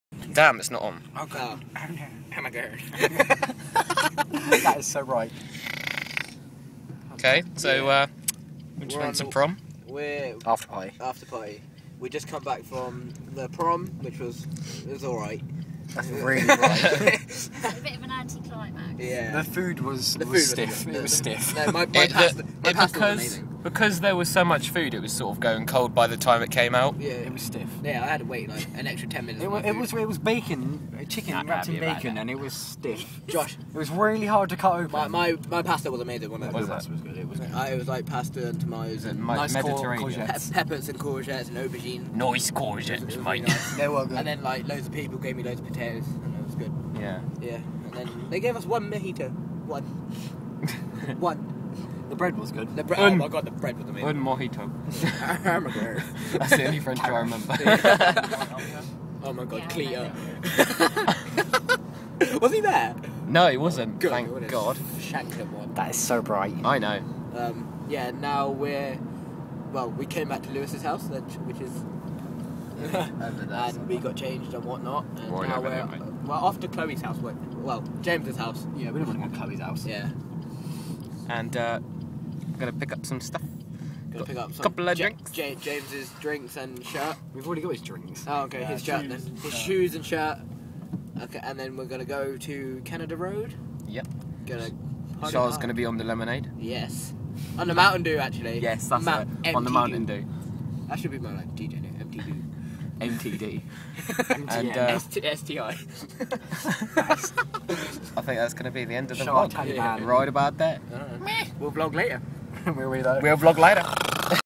Review | Botleigh Grange Prom
It was supposed to be a little vLog about what we were doing but it turned out to be a little bit of a review about the food at the Botleigh Grange hotel on our Prom night.